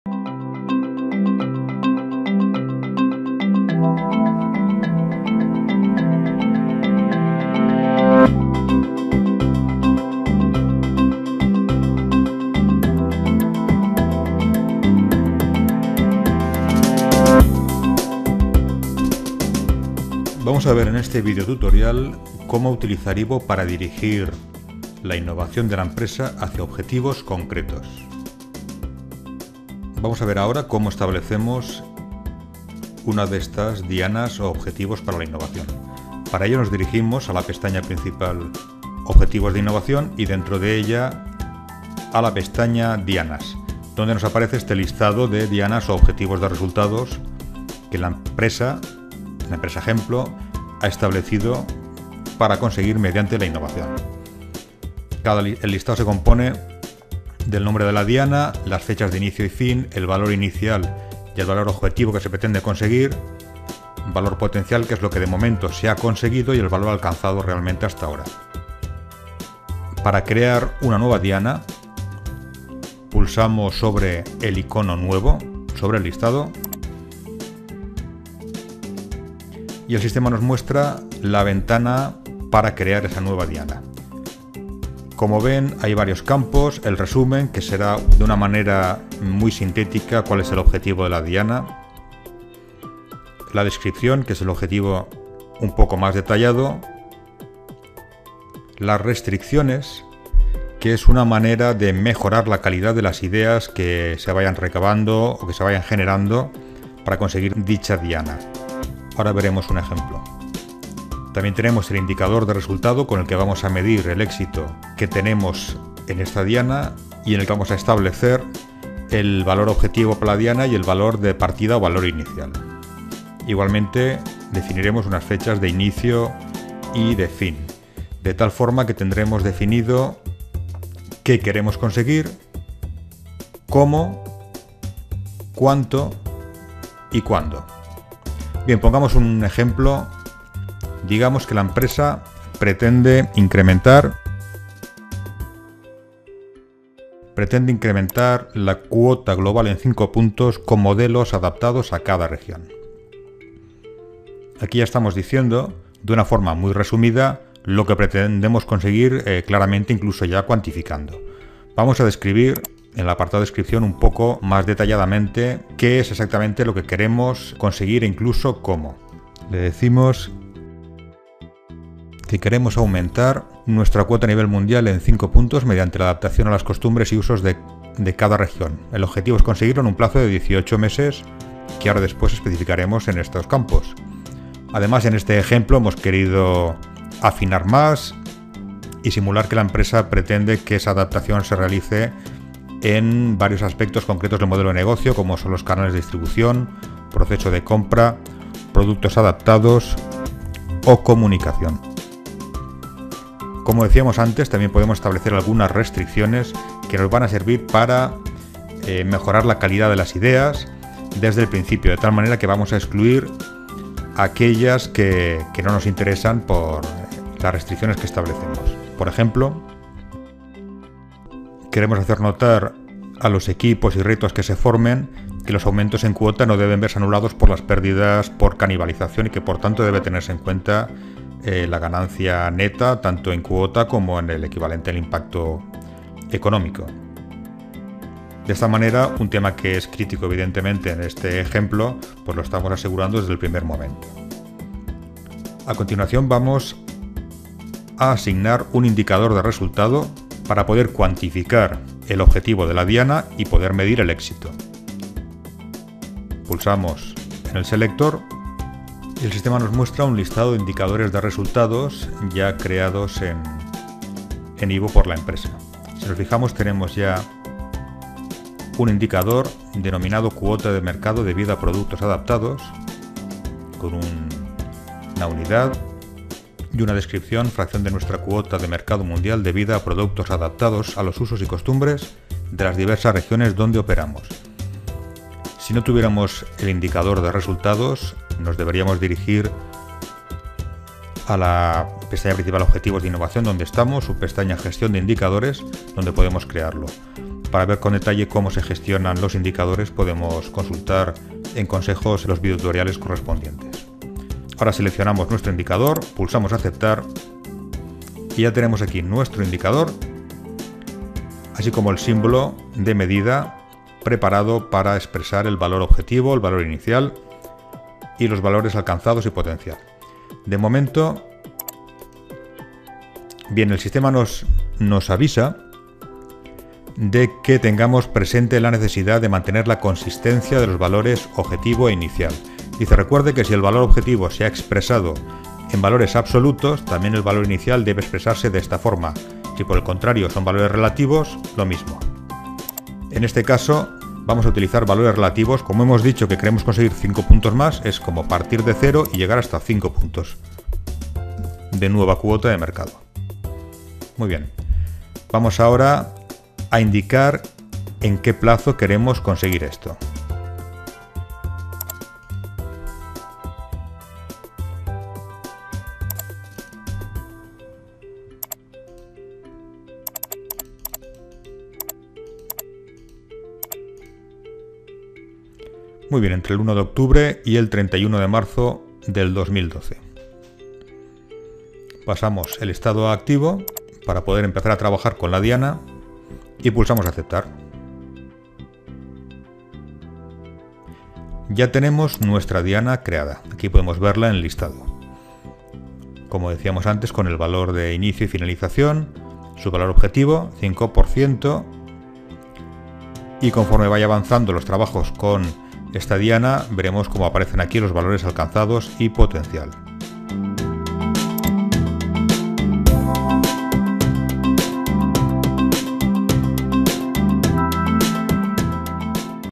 IbO tutoriales: Establecer un objetivo de innovación
IbO tutoriales: ¿Cómo establecer un objetivo a conseguir mediante la innovación?

Asegure desde el principio que las ideas le conducirán donde su compañía desea ir. La calidad de la Innovación se multiplica cuando sabemos dónde queremos llegar con ella.

Con IbO podrá comunicar a su organización objetivos claros y tangibles con los que las personas se pueden identificar. De esta forma obtendrá ideas más enfocadas y rentables, y un mayor compromiso con la innovación.